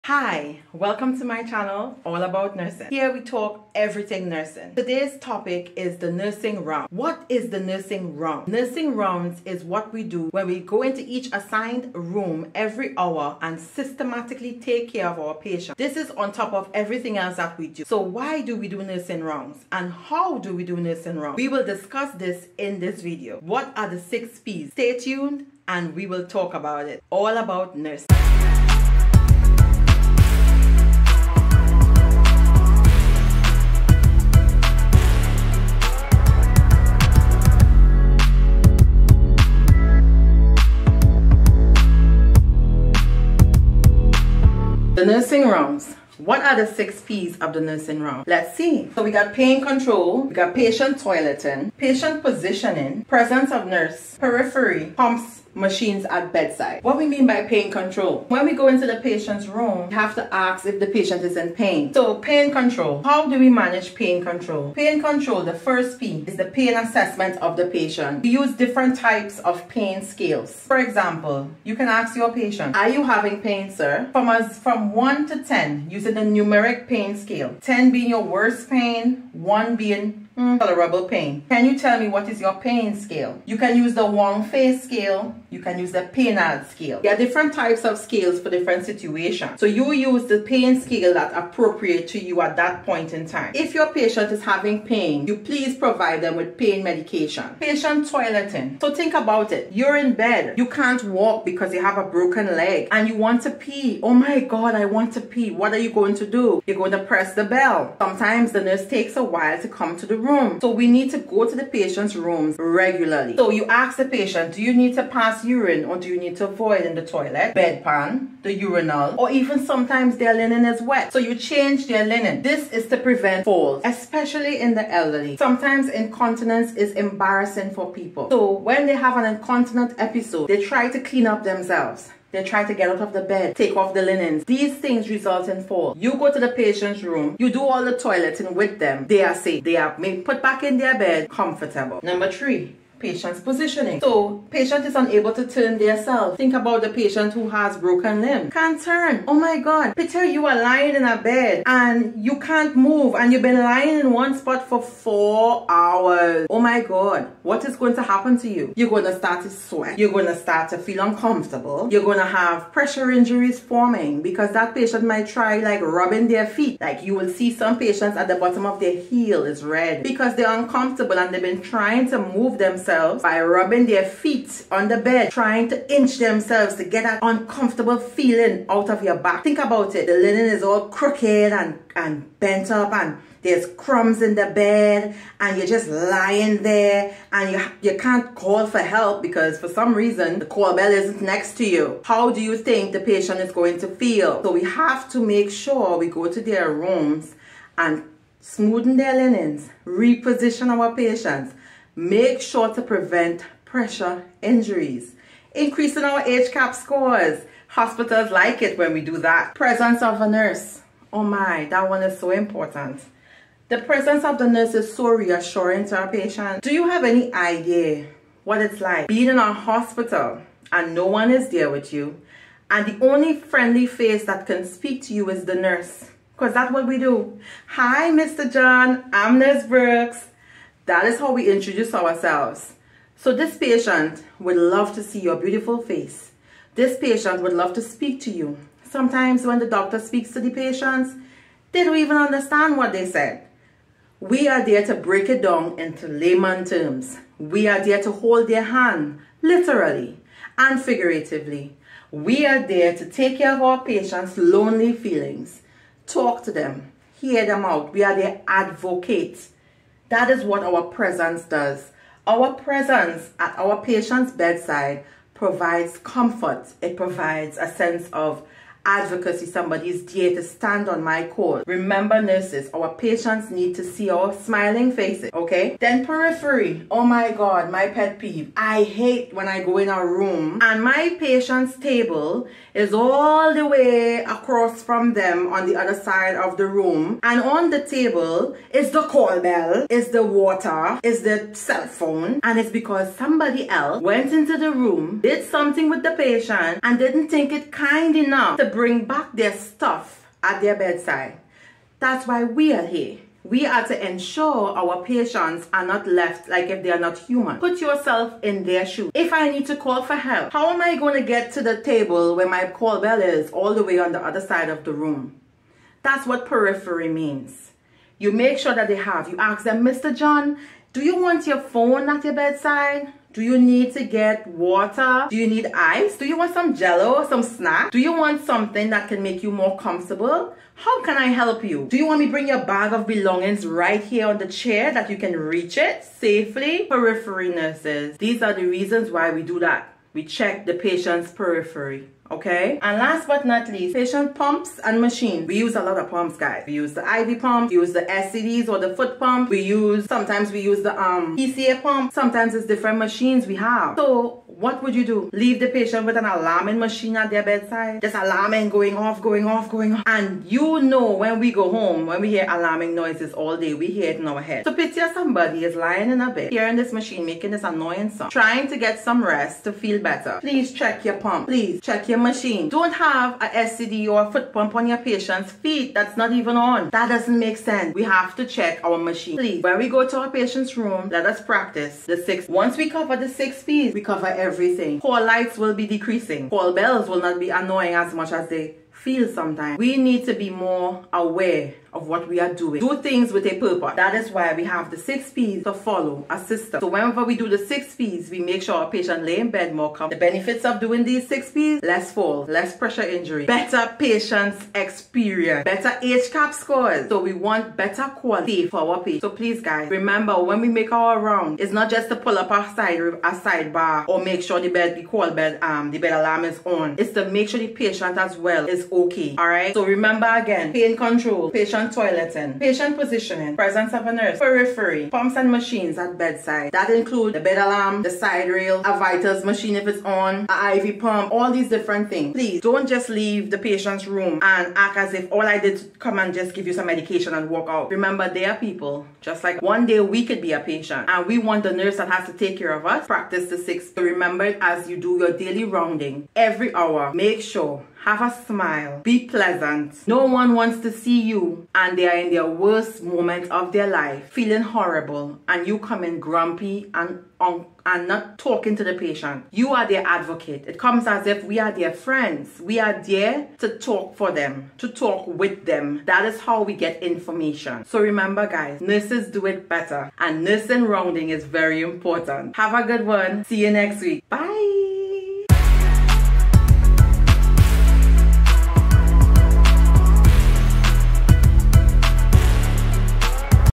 What are the 6 P's Of Nursing rounds
HEY guys in this video we are talking about the nursing rounds . As nurses we know that  there are multiple different types of rounds. But today we are chatting about the hourly nursing rounds . We are going to use the rule of 6 P's.  This video is bring education to the public on what we do as well as bring back the love an the passion for nursing . We are nurses we do it better .

Lets love being a nurse again !